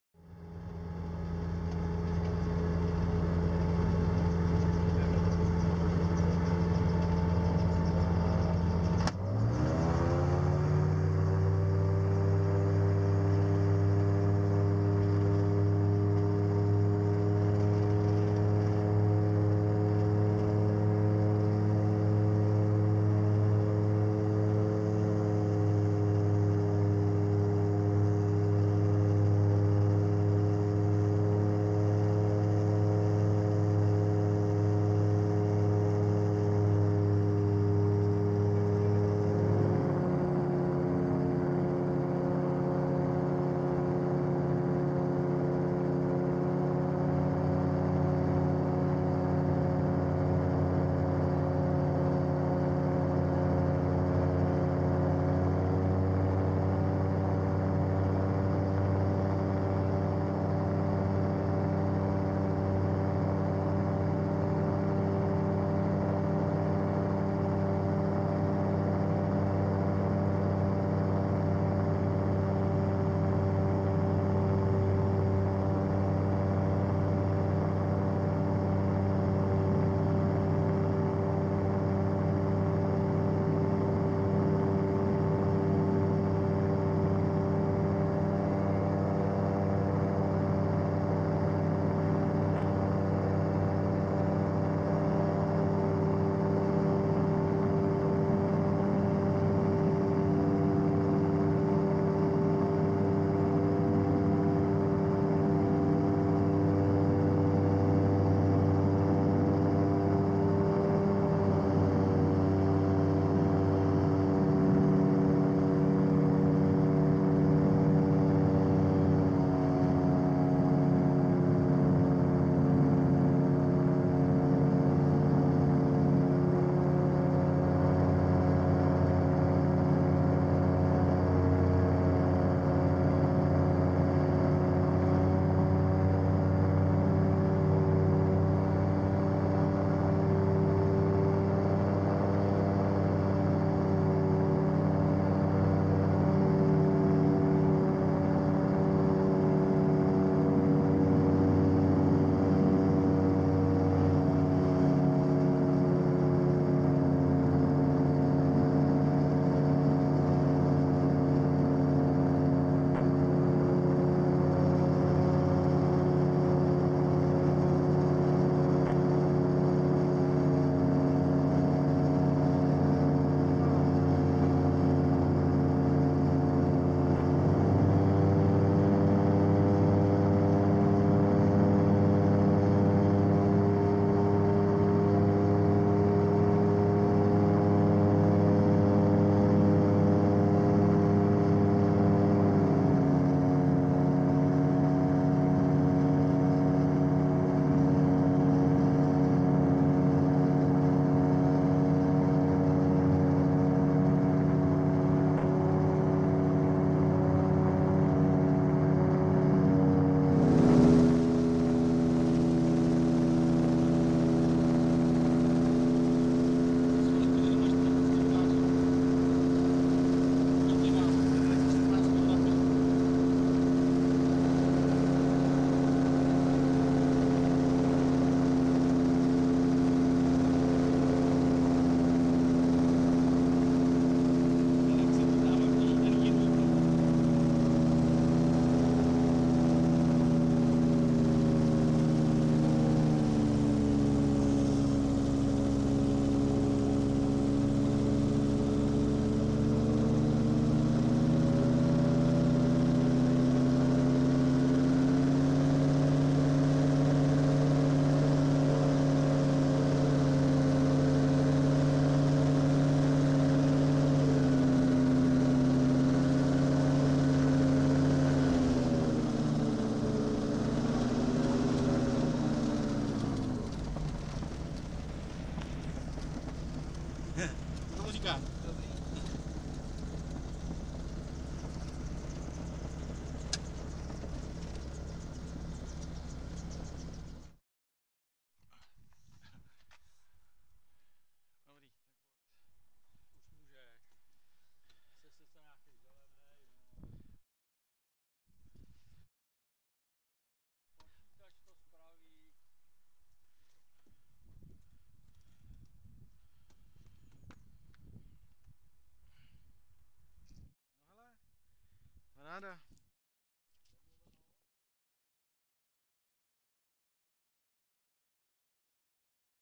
Pekelný ostrov 2016 a hrachy - letecké záběry
Jak to vypadalo po prvním večeru na pekelňáku jsme vám se Slávkem natočili :) ... a přibalili k tomu  pár záběrů z přehrady Hracholusky :)